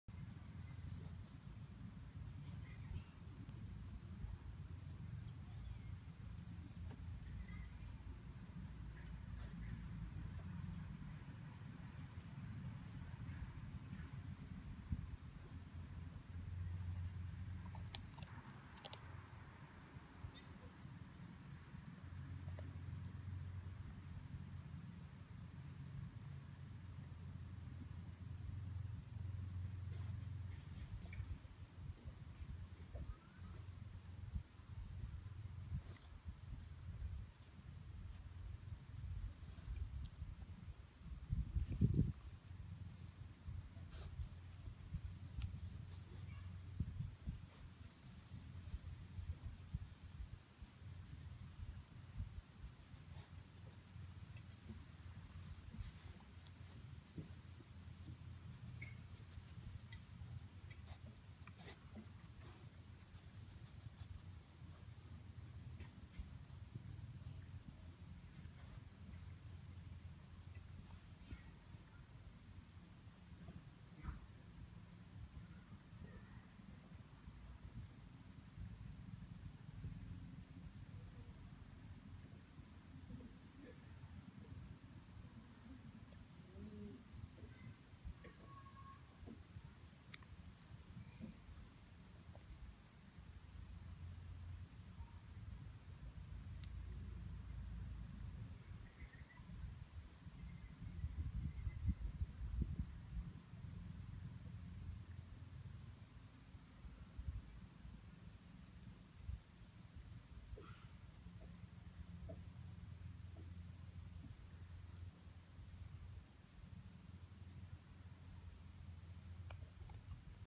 Pippi seeking food
Pippi seeking her food on grass. This is absolutely mindfulness for dogs and calms your dogs nervous system... back to nature :)